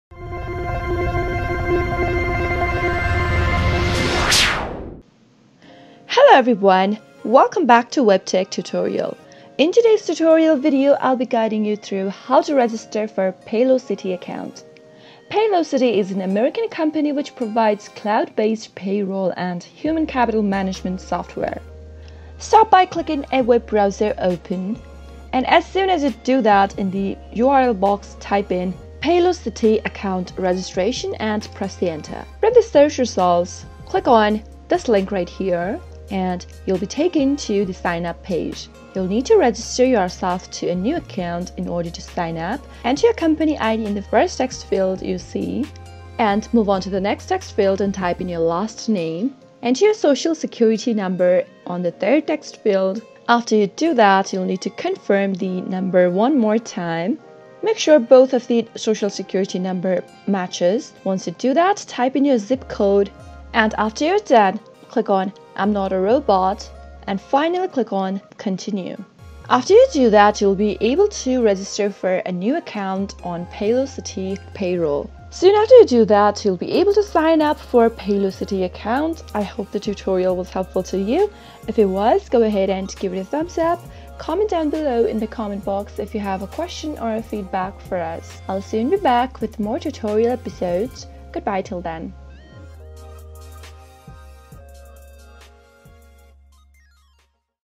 Paylocity Account Sign Up 2021: How to Register/Create Paylocity Account?
Register yourself on Paylocity online account. Learn how! Here in this video we are going to guide you through step by step process on how you can easily sign up on Paylocity, just by following a few simple steps.

Paylocity is an American company which provides cloud-based payroll and human capital management software.
The Company provides human resources services such as payroll, health benefits, retirement plans, time and labour, and workers compensation, as well as tax filing, recruitment, screening, employee management training, and other additional services.
Register yourself on Paylocity to get access on the management of all the services.
Follow the steps below to sign up on Paylocity online Account:
1: Go to your web browser and type in ‘Paylocity registration’ and click on the link shown in the video
2: Enter the Paylocity assigned Company ID.
3: Type in your Last Name.
4: Enter the nine digit Social Security number in the SSN field and enter the SSN a second time to Confirm
5: Enter the Home Zip Code and Click on the Continue button.
6: Enter the Username.
7: Finally create an Password and you will be registered in Paylocity